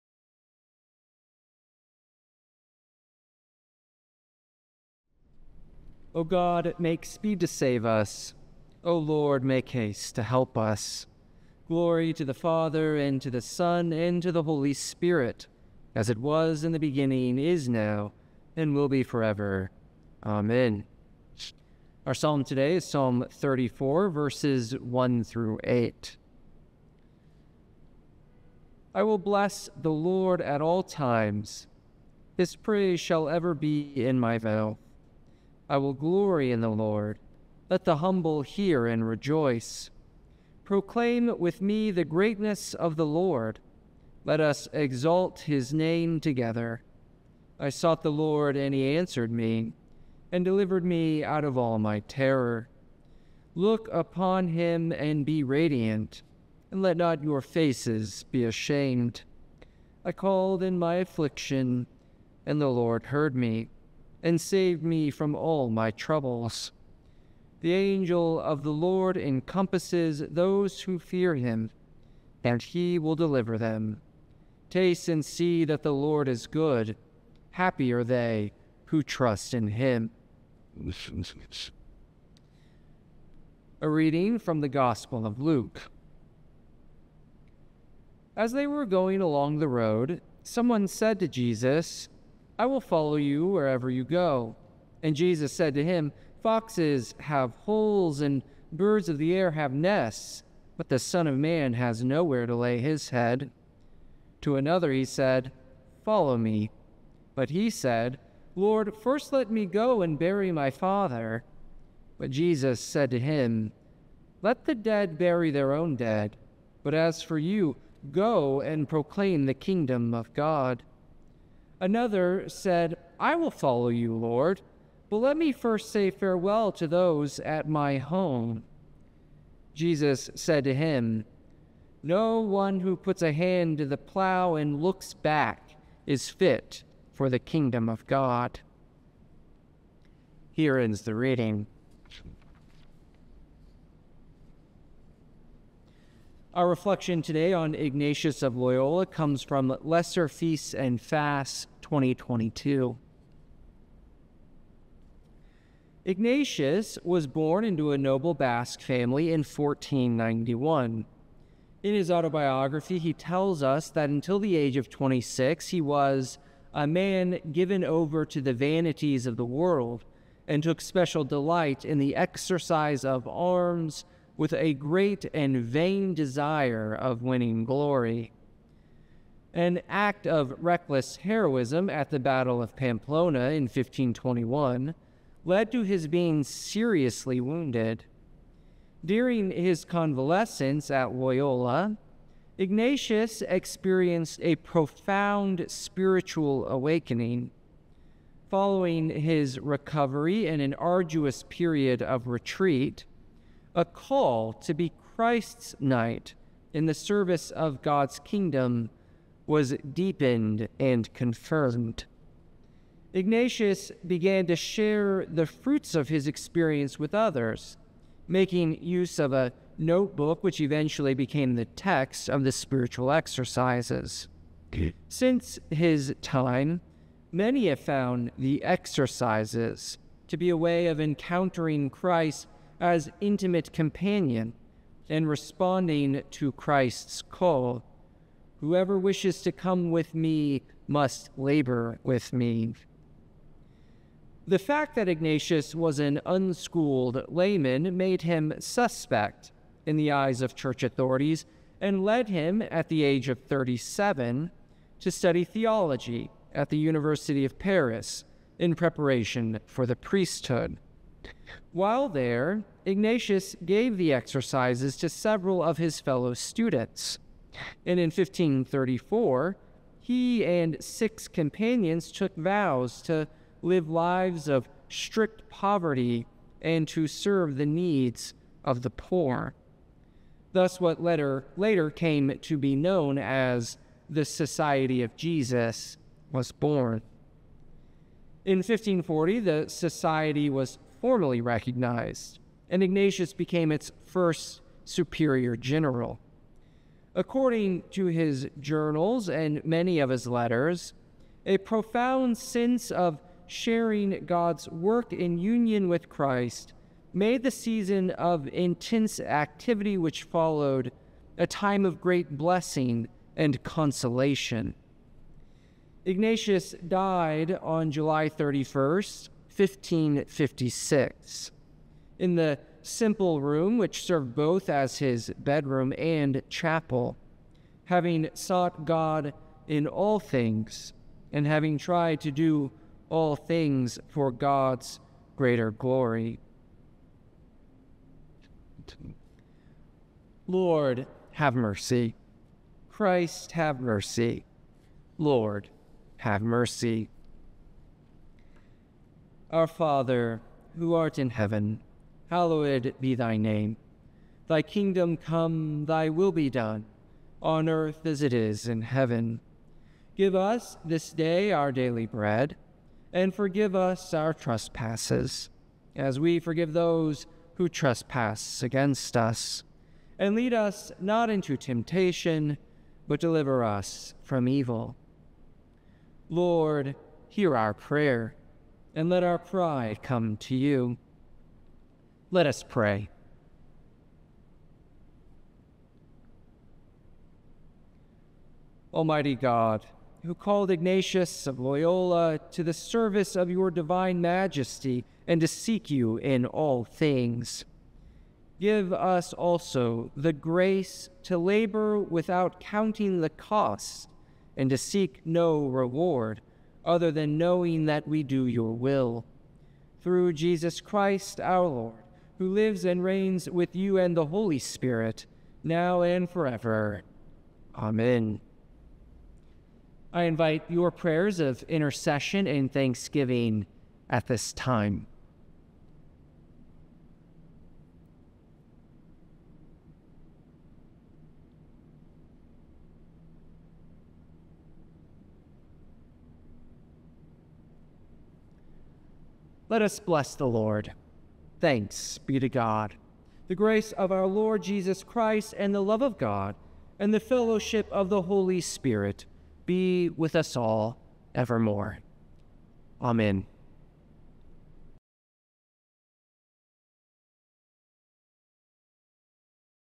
Noonday Prayer, July 31
Welcome to Noonday Prayer for July 31, 2024, The Lesser Feast of Ignatius of Loyola, Priest and Spiritual Writer.

The service begins on page 103 in the Book of Common Prayer.